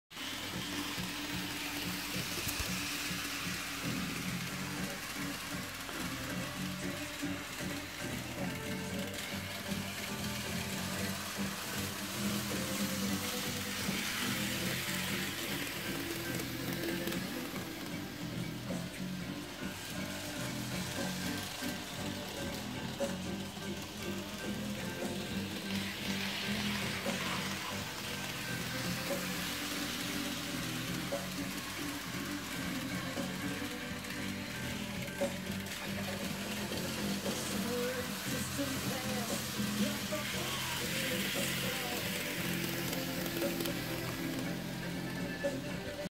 MP36-PH Metra - N Gauge
Kato N Gauge Metra MP36-PH propelling a 3 car set.